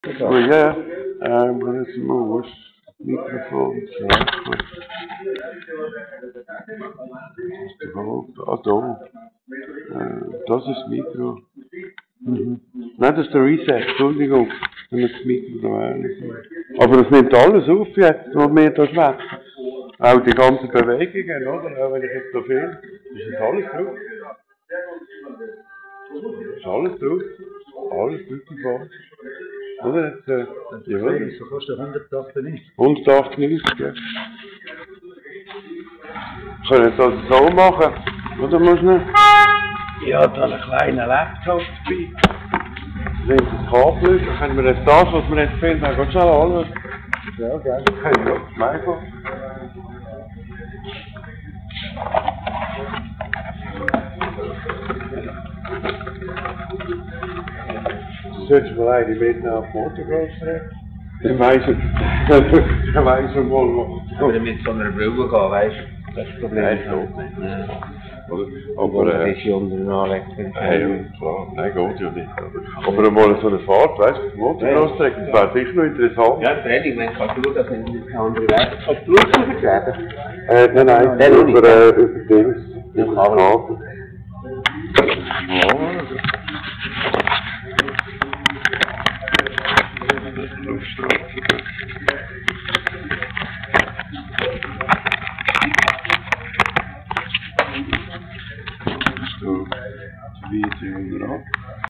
blabla.avi
Aufnahme mit 3mio Pix.
Wirre Bilder, aber nettes Gespräch im Hintergrund.
Beim Montieren und Erklären der Kamera.